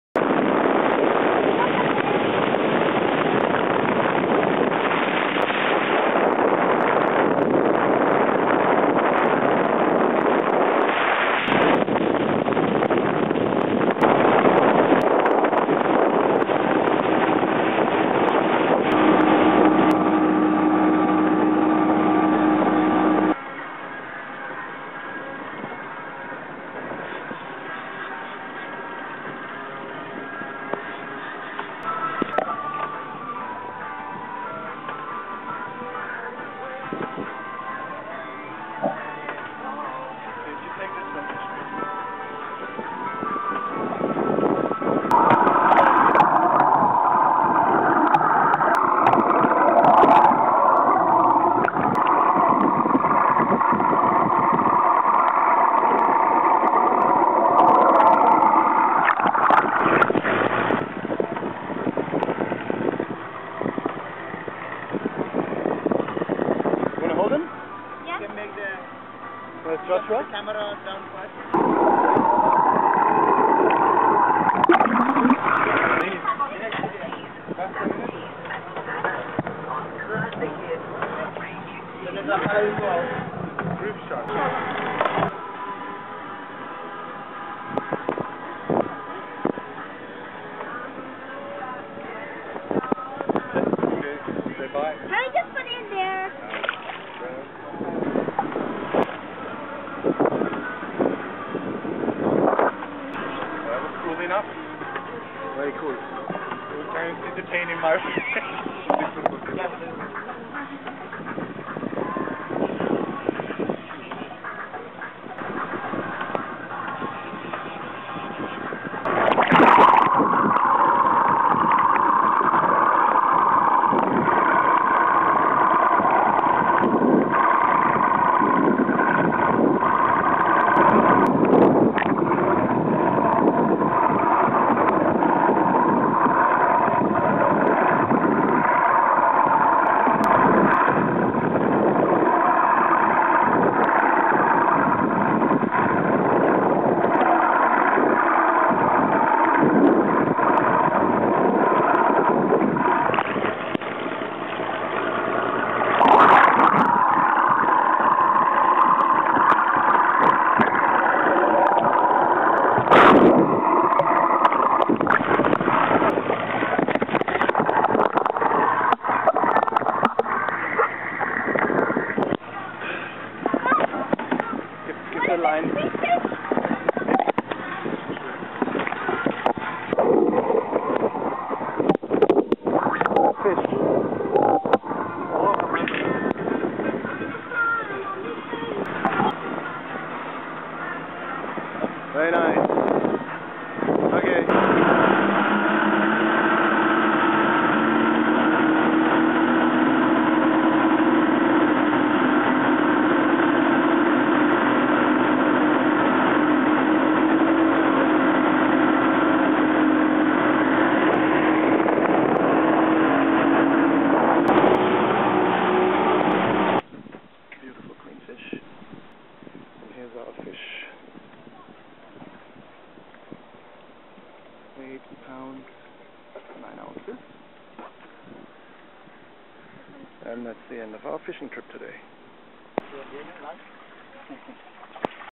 Fishing Adventures #20 - Catching Baby Shark and Queenfish in Abu Dhabi
Deep sea game fishing trolling in Abu Dhabi UAE (not Dubai Fujairah Oman Kuwait Bahrain or Doha) and catching baby shark and queenfish - new underwater video footage. Please rate us and buy us a coffee! FISH ON!